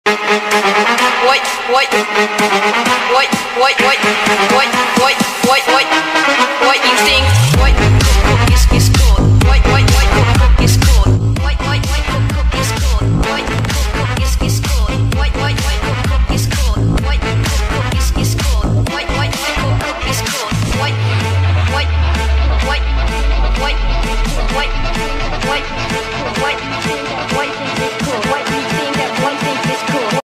Have you seen this kind of dredging scene
This is a picture of an amphibiousexcavator dredging. Some people think that people who drive this kind of amphibious excavator also need to be able to dig a dredger. Some people think that the modification of amphibious excavators should pay attention to reasonable design.
🤔What is your opinion on amphibious excavators?